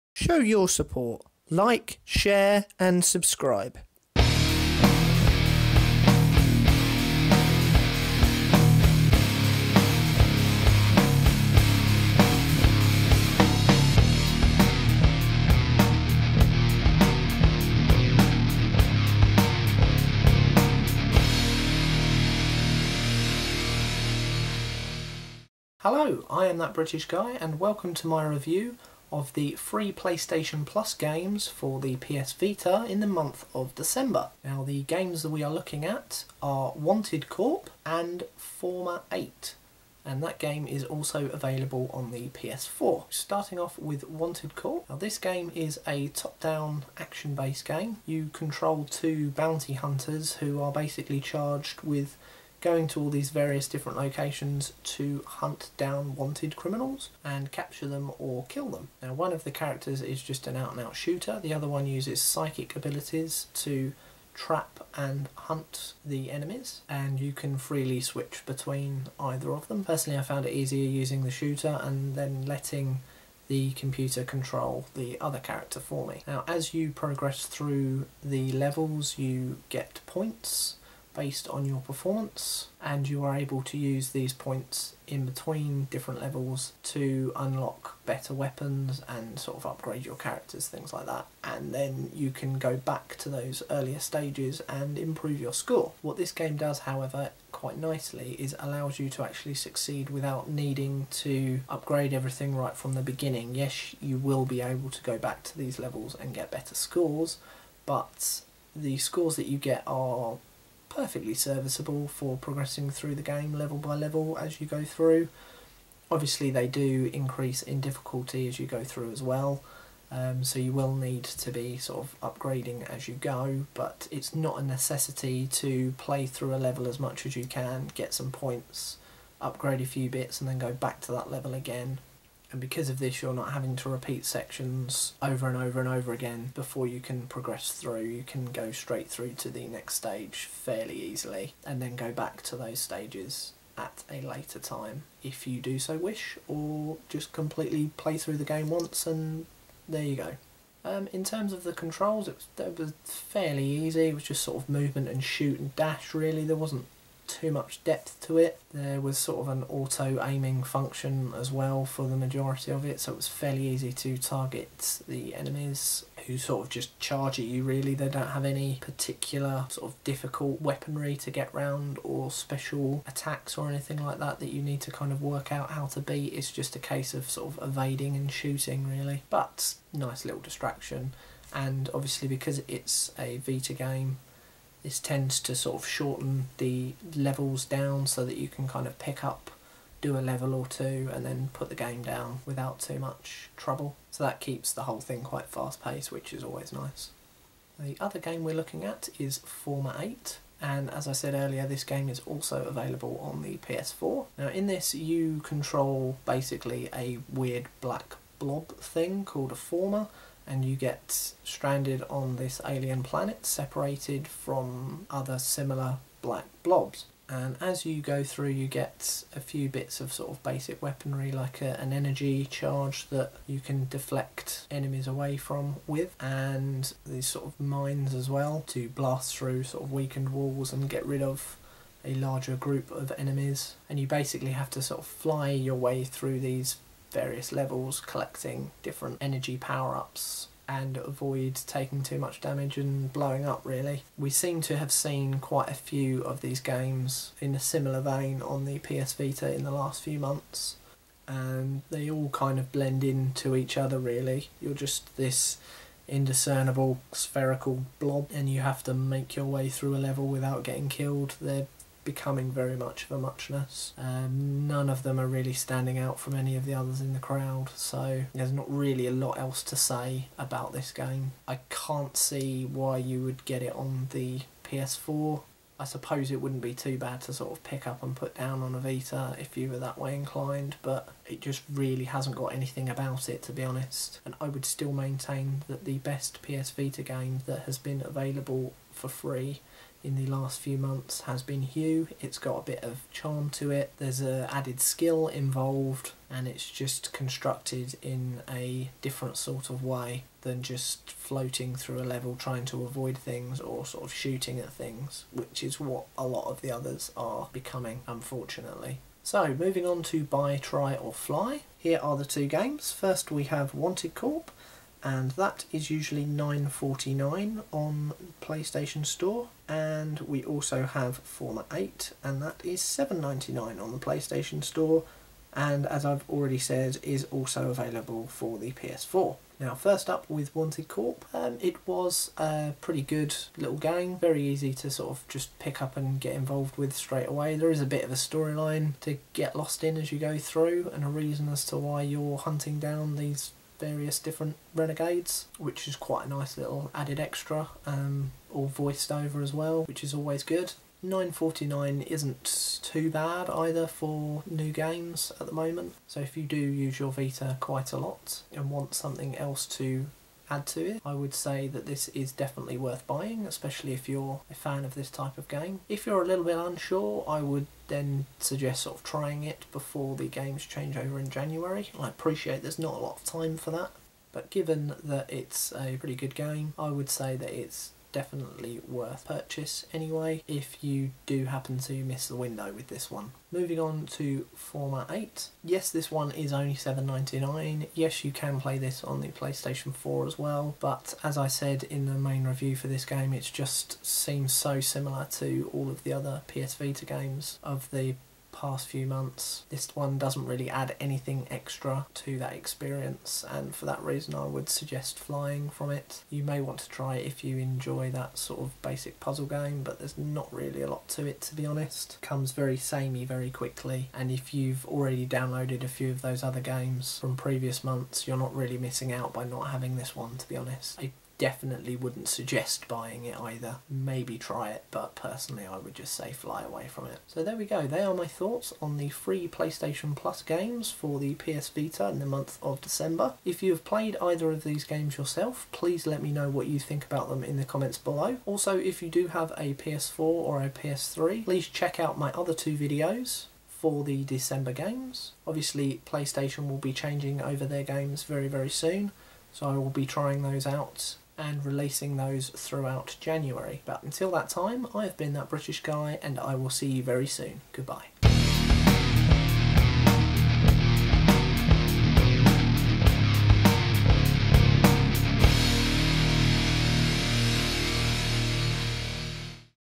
PS+ Free PSVita Games for December Review
Here is my final review of 2017 (yes I know this is going live in 2018).

This is my review of the 2 PS Vita games free to Playstation Plus Subscribers, Wanted Corp and Forma.8.